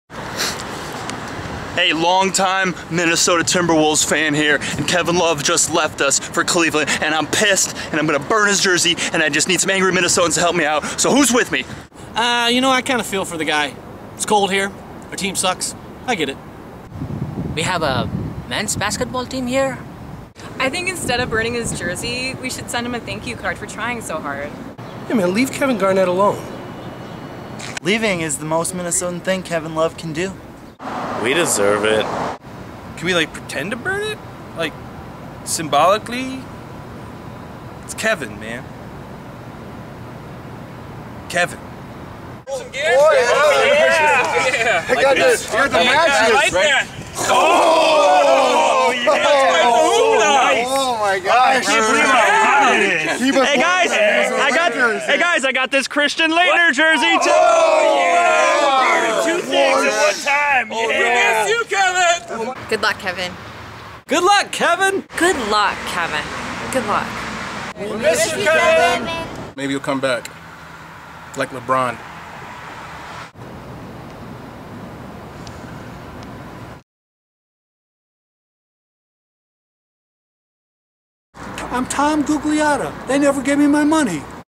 Kevin Love Jersey Burning - Minnesota Nice Style
Written by Mike & Zan Brody. Edited by Zan Brody.

Starring Mike Brody, Angelo Vescio, Senthil Rajasekharan, Elizabeth Ess, Gus Lynch, Sidney Oxborough, Justin Colucci, Mike Rasmussen, Mike O'Keefe, Darrin Chase, Earl Elliot, Jenn Schaal, Tim Messenger, Raleigh Weld, Alvin Irby and Ali Sultan.

Thanks for the six years, Kevin!